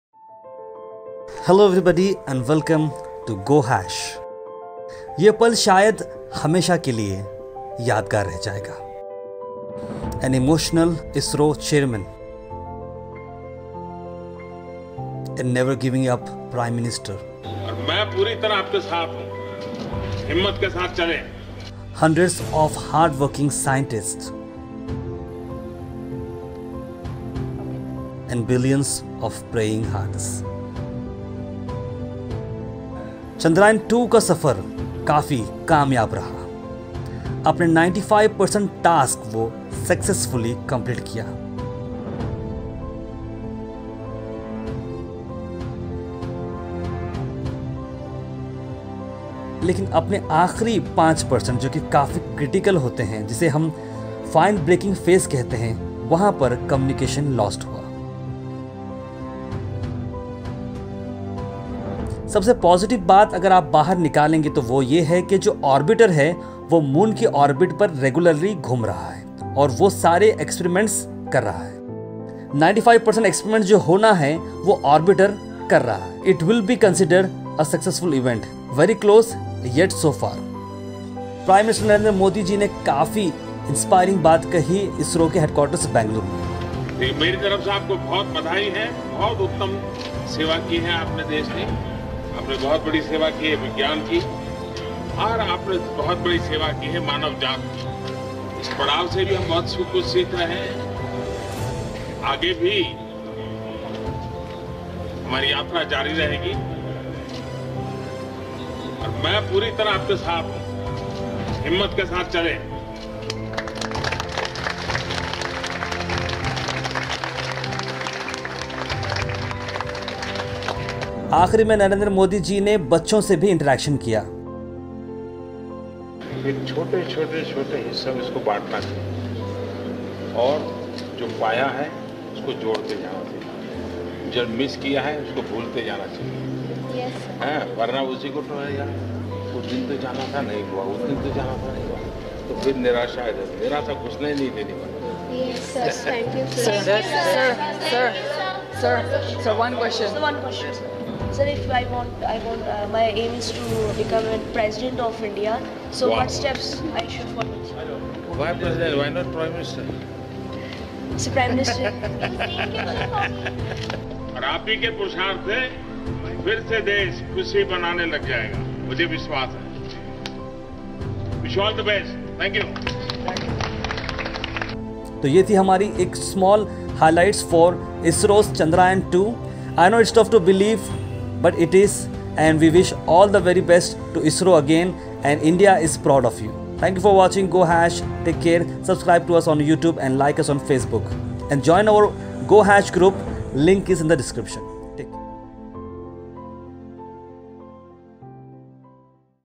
Must Watch !! Emotional Moments of ISRO Chandrayaan 2 @ ISRO,
MUST WATCH !!! Emotional & Inspiring Moments of ISRO Chandrayaan 2 @ ISRO, The Complete Highlights by gohash.in Source:DD national.